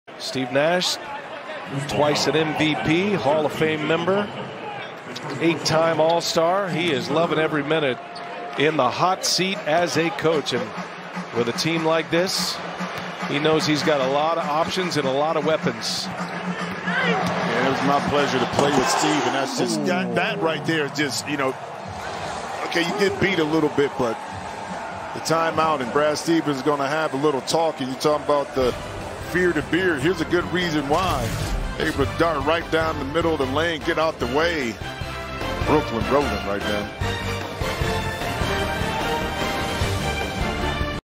真一哥！ 大鬍子 James Harden 突破五人防守單手扣籃｜塞爾提克 vs 籃網 第五戰 2020-21 賽季 季後賽
Boston Celtics @ Brooklyn Nets 6/2

喜歡的話幫我衝個1000訂閱喔！ 感謝各位